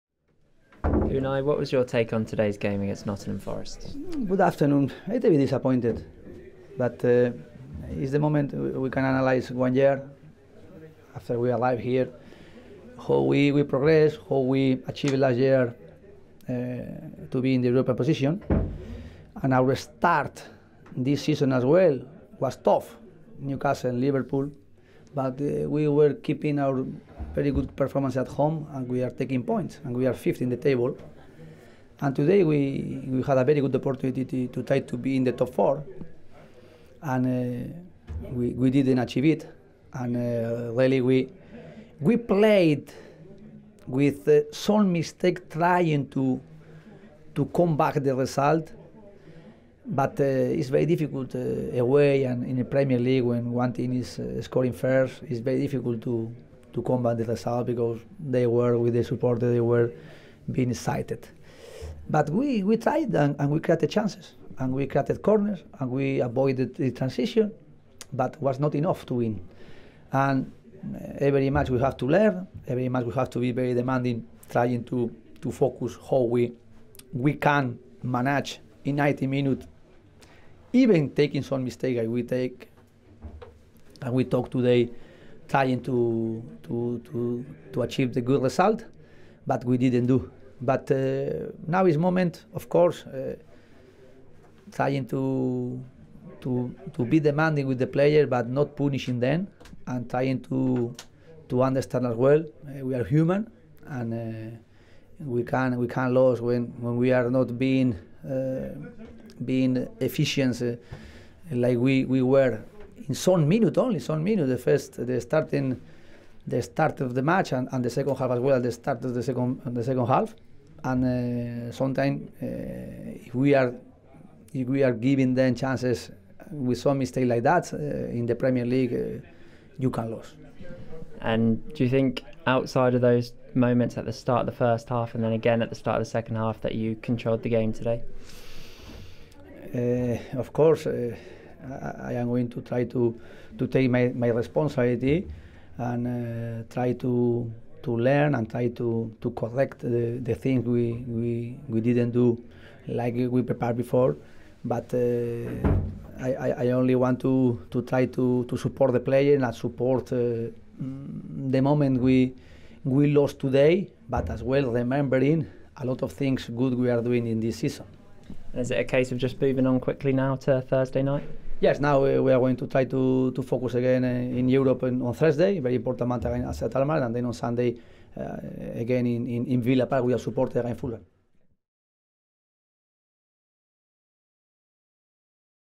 POST MATCH | Unai Emery on Defeat to Forest
Unai Emery speaks with the media following Aston Villa's defeat to Nottingham Forest.